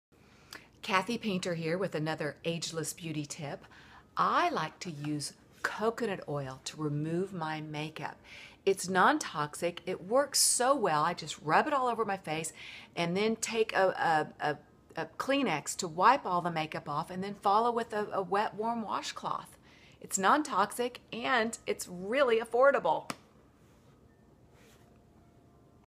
Coconut Oil: For Nontoxic Makeup Remover use Coconut Oil
Coconut Oil makes a fabulous make up remover.  for more info on coconut oil.
Coconut oil is nontoxic, anti bacterial, anti viral, anti fungal.